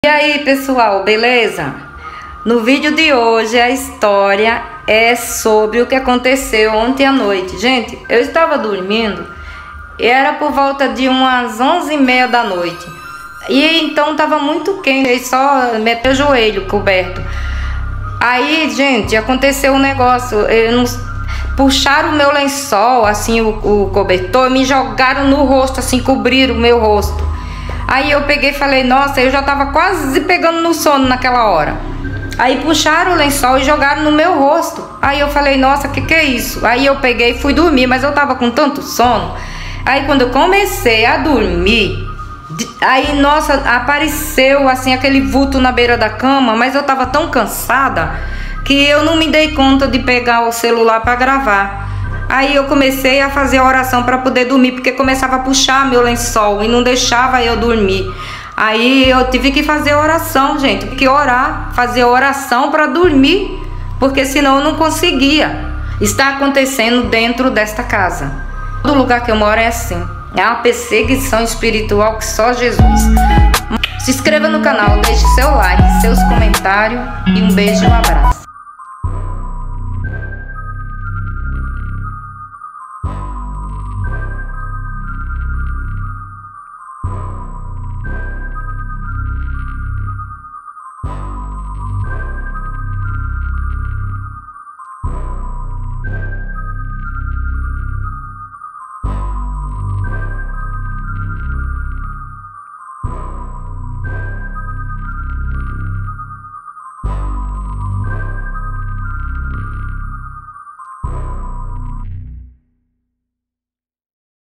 O Mistério Sob o Lençol - Algo Sobrenatural na Minha Cama!
"Prepare-se para uma jornada assustadora através do mundo dos sonhos e pesadelos! Neste vídeo, testemunhe eventos sobrenaturais surpreendentes que ocorrem enquanto eu durmo. Enquanto durmo tranquilamente, o inexplicável se desenrola diante de seus olhos. Espíritos inquietos, visões aterrorizantes e fenômenos paranormais enchem o ambiente, transformando a noite em um verdadeiro pesadelo. Acompanhe-me nesta experiência arrepiante e descubra os segredos ocultos que se revelam durante o meu sono. Esteja preparado para sentir calafrios na espinha e questionar a realidade enquanto mergulhamos em um mundo onde o inexplicável se torna tangível. Você ousa enfrentar os mistérios do sobrenatural enquanto durmo?"